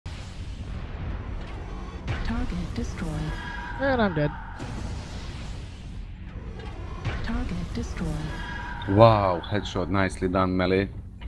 Dual Headshot :)
Here is a nice short video of me scoring 2 head shots one right after the other hehe :)

Didn't know I scored a headshot on that Timber until later on in the fight when Dr Angst confirmed it was one as well... Just knew I did score a headshot on the battlemaster.

Current headshot count... 169!